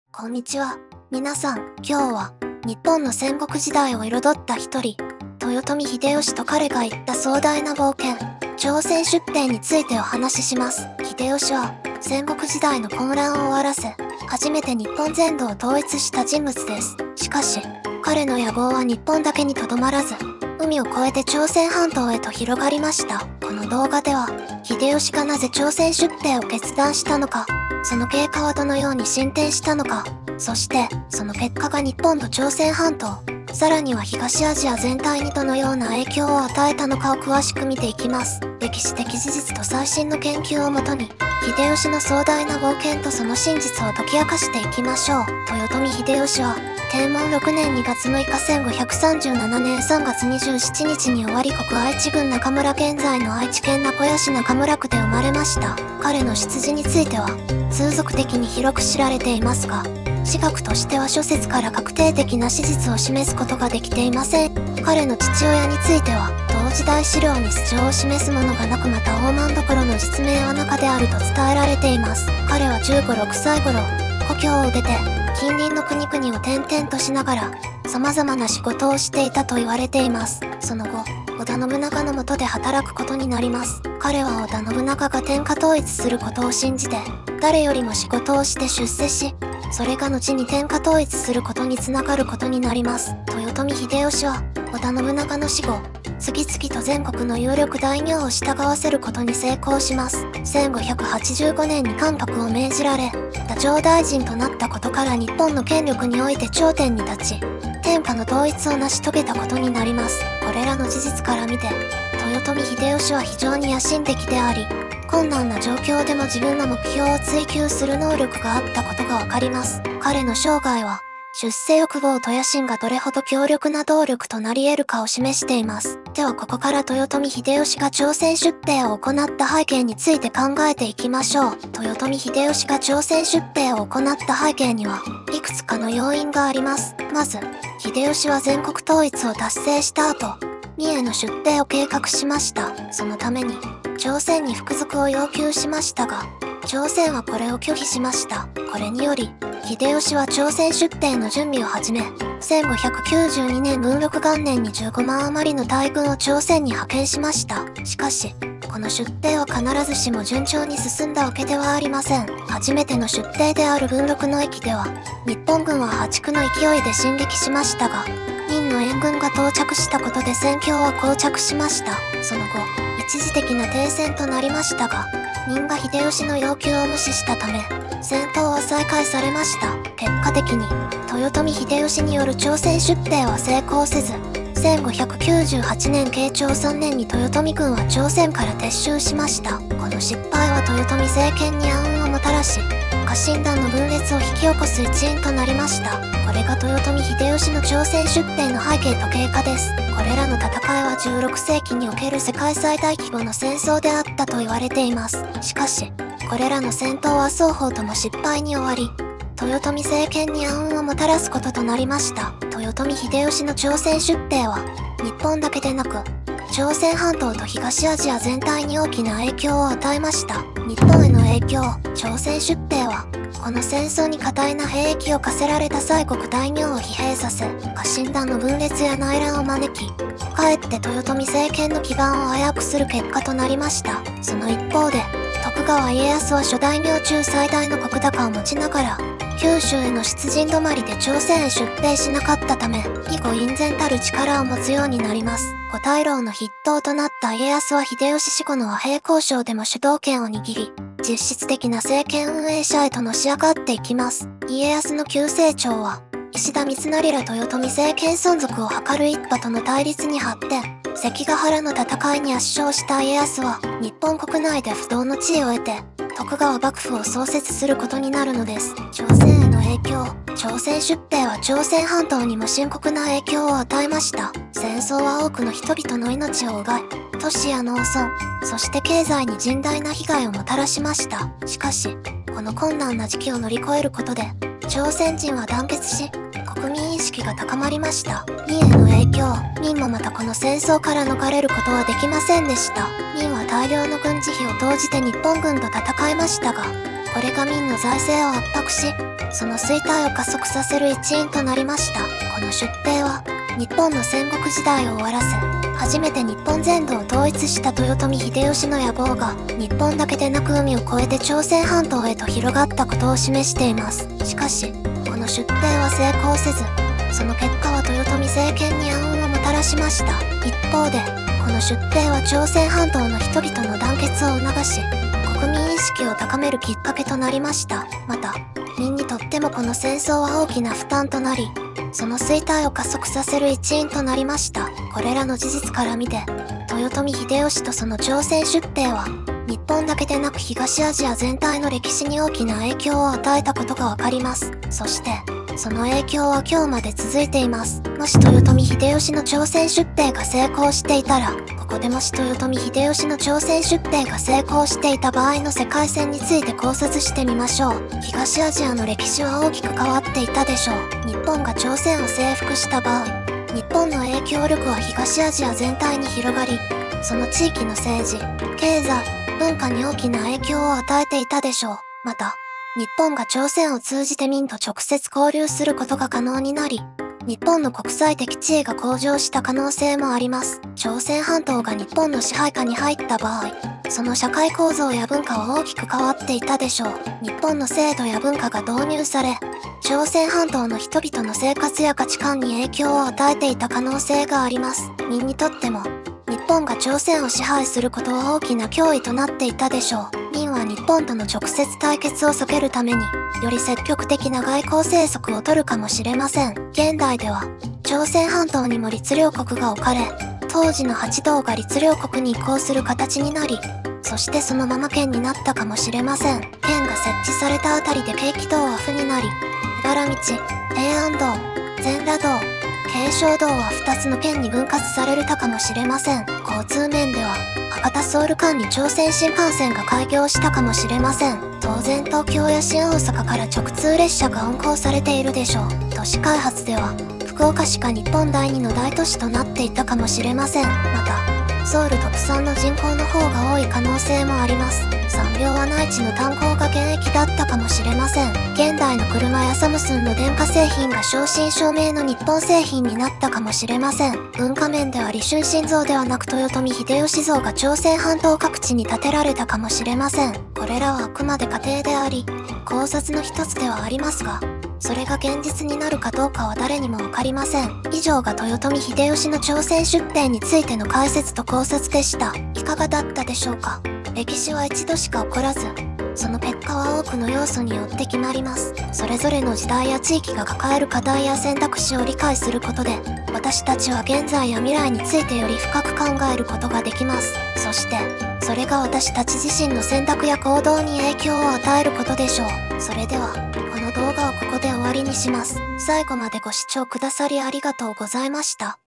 豊臣秀吉の朝鮮出兵：歴史的解説と影響
この動画では、豊臣秀吉の朝鮮出兵につい独自の視点で解説します。日本の戦国時代を終わらせ、初めて日本全土を統一した豊臣秀吉の野望が、日本だけでなく海を越えて朝鮮半島へと広がりました！
どのような結末が待っているかご覧下さい。